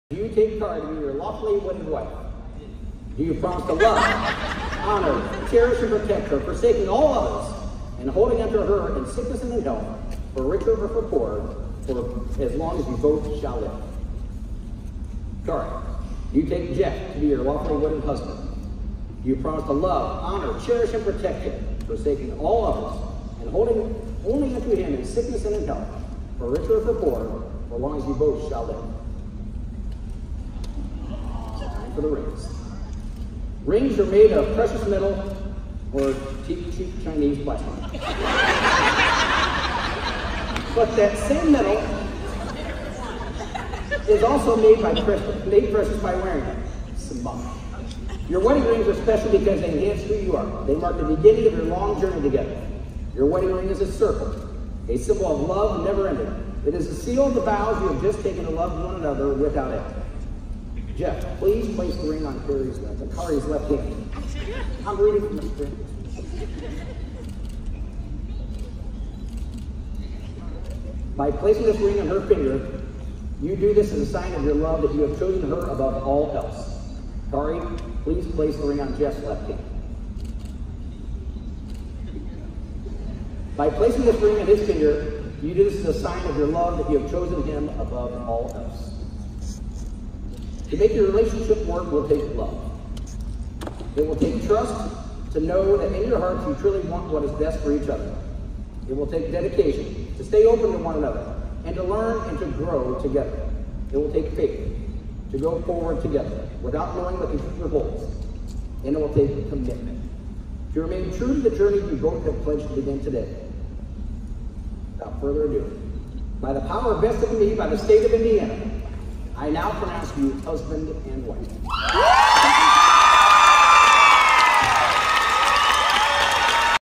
Lawrence North teachers tie the knot at school
Two Indianapolis teachers exchanged vows right in the high school gym before an audience of cheering students. Watch them get married in this sweet and funny short ceremony.